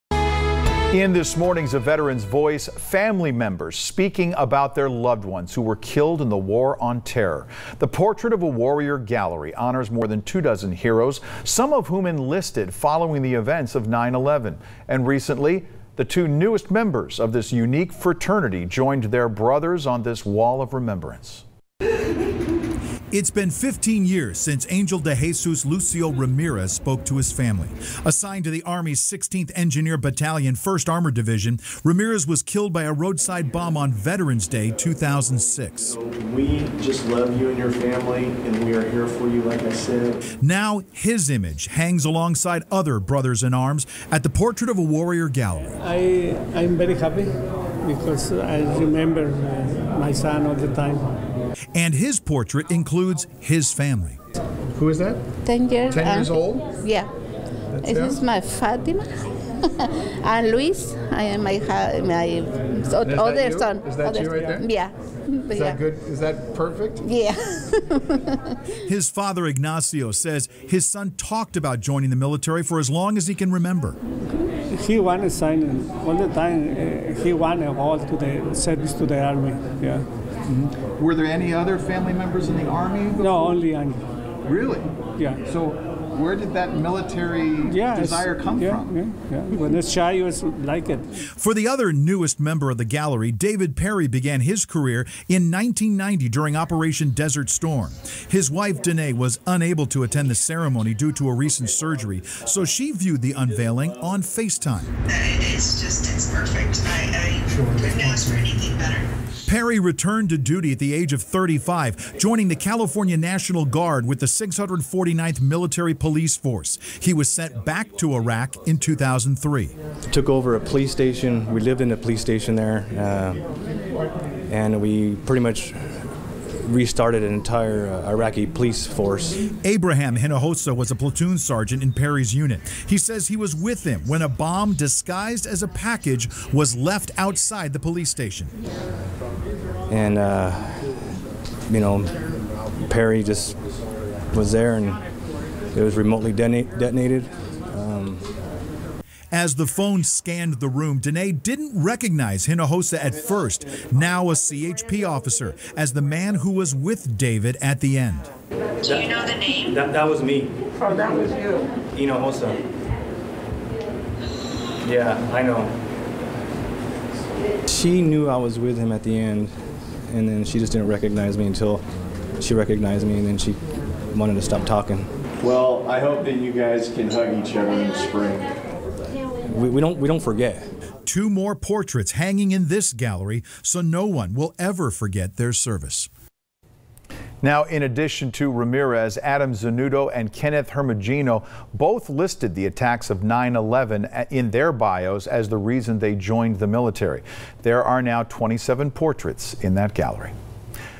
A Veteran's Voice: Portrait of a Warrior Gallery
The Portrait of a Warrior Gallery unveiled two new members to their special fraternity.  Angel Ramirez and David Perry remembered along with 25 others who gave their lives in the war on terror.  A moment to reflect on the 20th anniversary of 9/11.